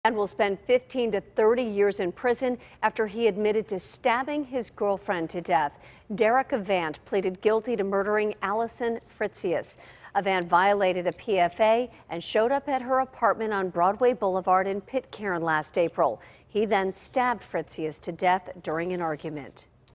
Man Sentenced To 15 To 30 Years In Prison For Murdering Girlfriend
A man will spend 15 to 30 years in prison after he admitted to stabbing his girlfriend to death.